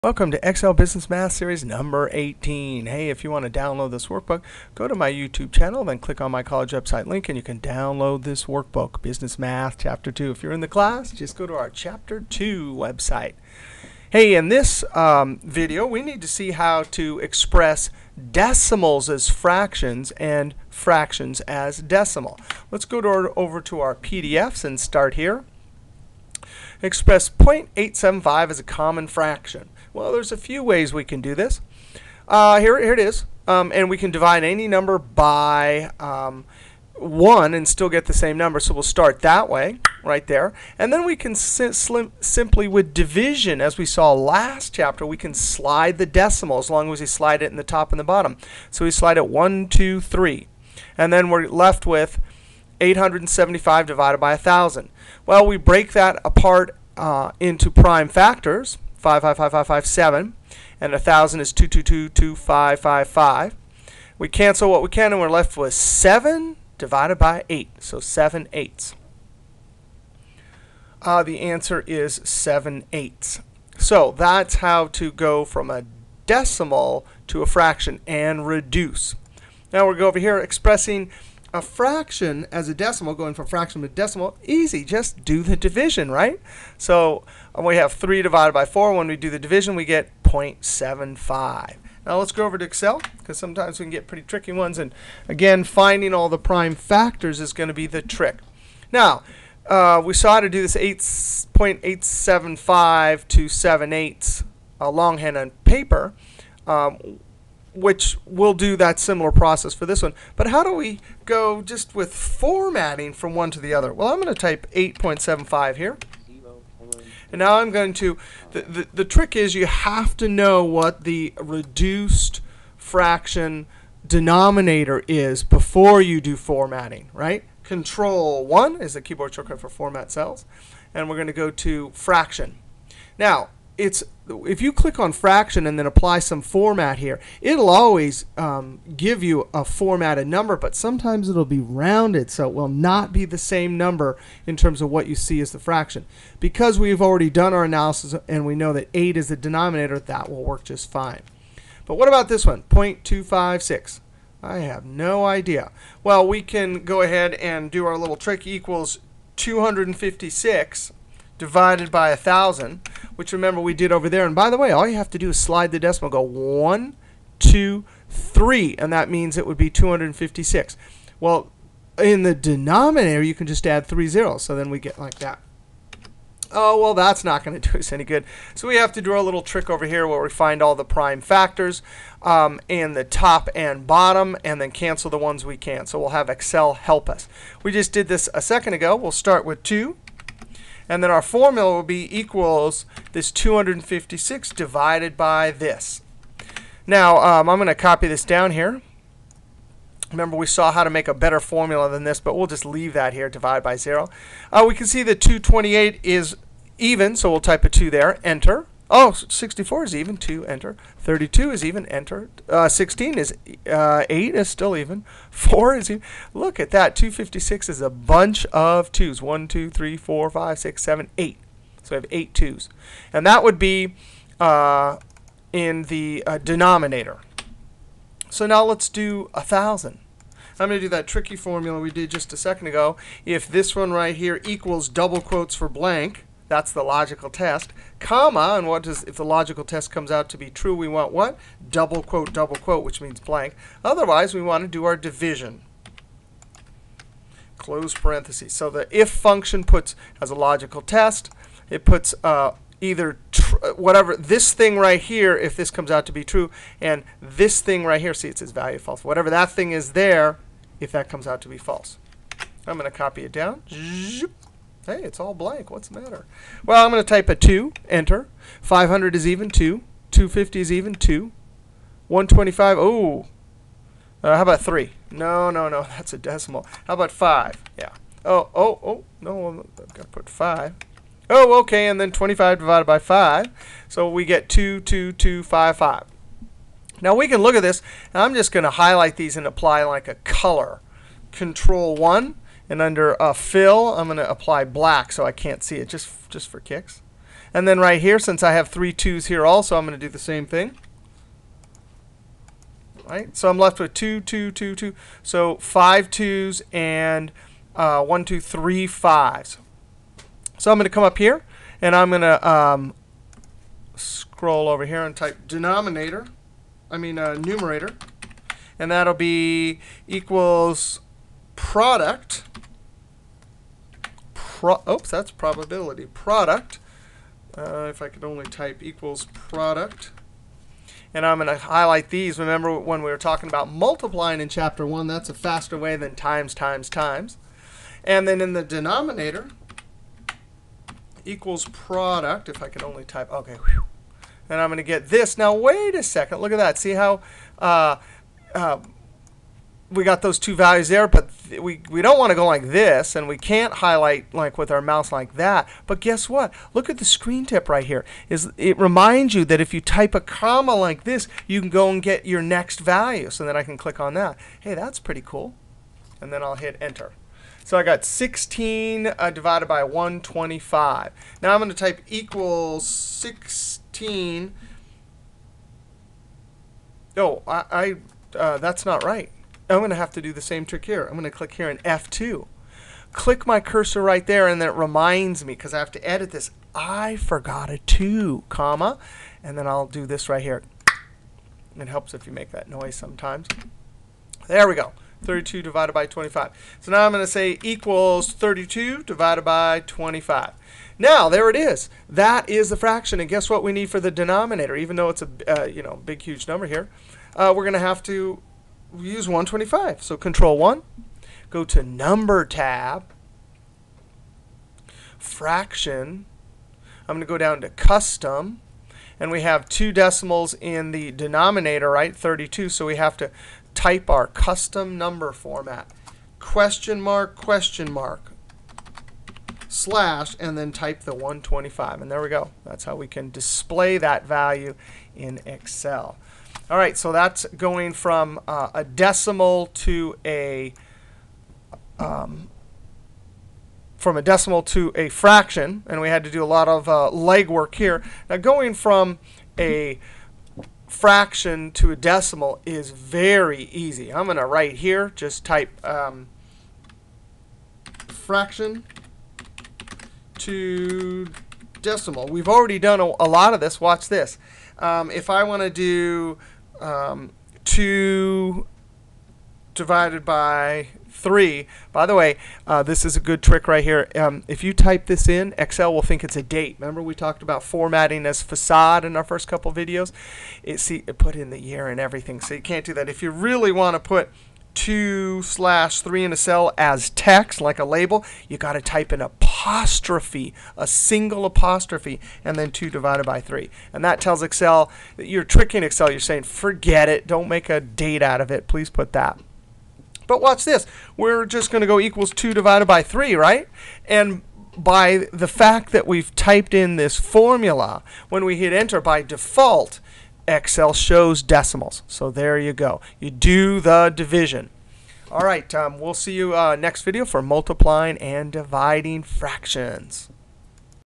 Excel Busn Math 18: Decimal Into Fraction
See how to go from a decimal to a fraction
See how to go from a fraction to a decimal